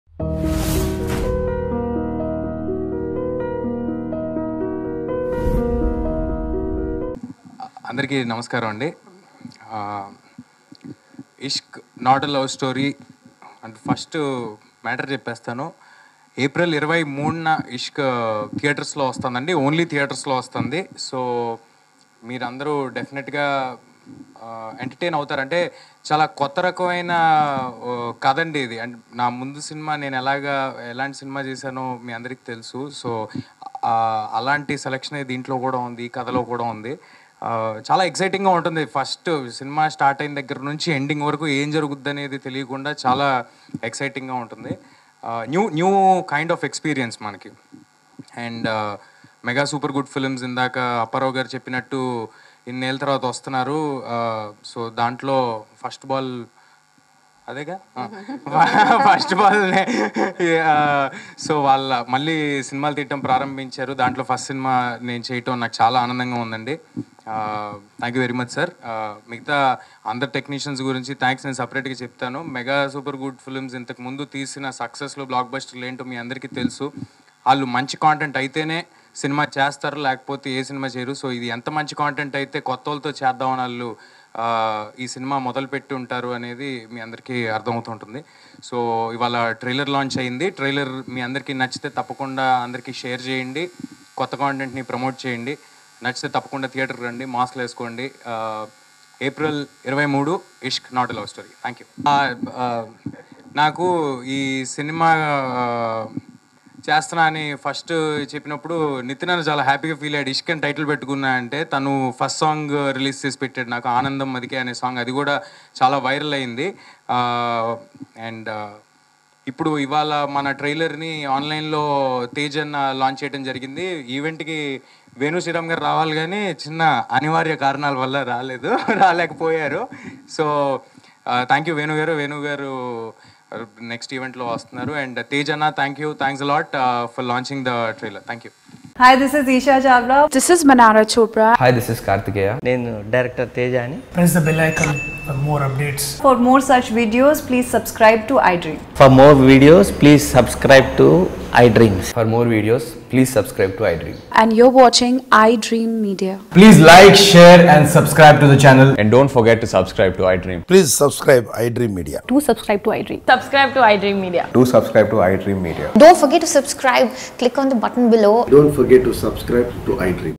Teja Sajja About Nithiin's Reaction On Ishq Movie Title | Trailer Launch Event | iDream Filmnagar
Watch Teja Sajja About Nithiin's Reaction On Ishq Movie Title.
After a long gap of seven years, RB Choudary and his Mega Super Good Films are back to Telugu cinema. Taking to social media, the popular production house announced their first film of 2021 and it is titled as ‘ISHQ’ with the caption ‘Not a Love Story’. Starring child actor-turned-hero Teja Sajja and Malayalam ‘wink’ sensation Priya Prakash Varrier as the lead pair, the youthful film is directed by SS Raju with music composed by Mahati Swara Sagar and camera handled by Sam k. Naidu. Details regarding the supporting cast and shoot are still awaited.